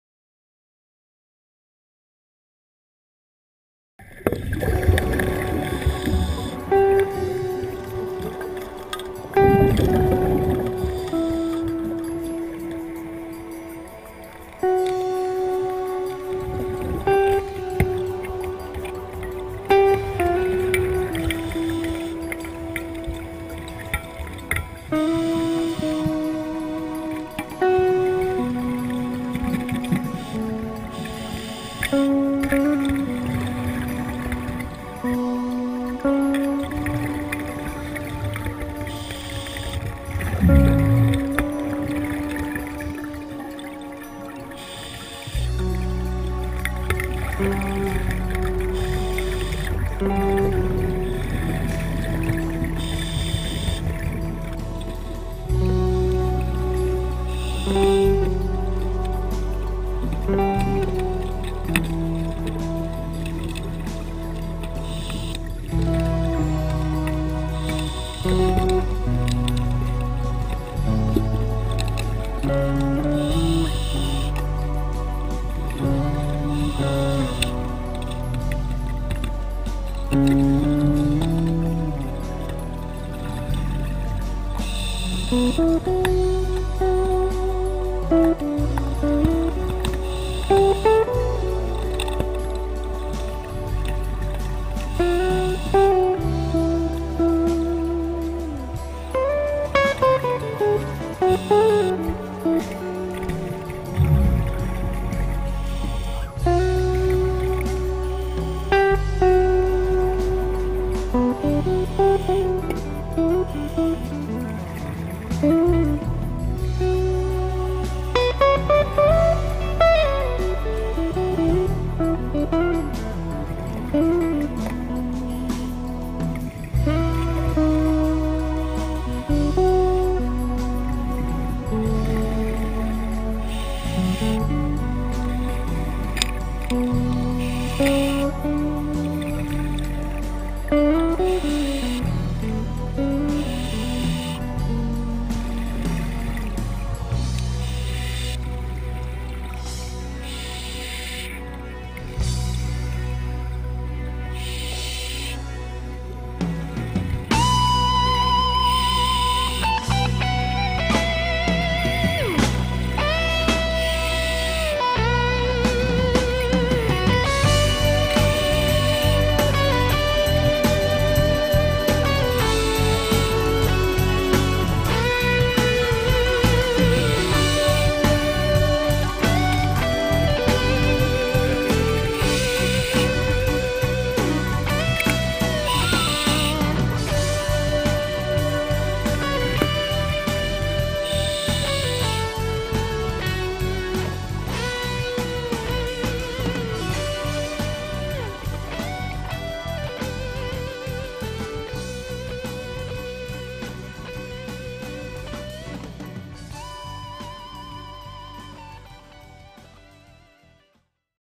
Chaudanne de 0 à 40 m
Plongé au recycleur de 0 à 40 m. C'est étroit, surtout avec un scooter.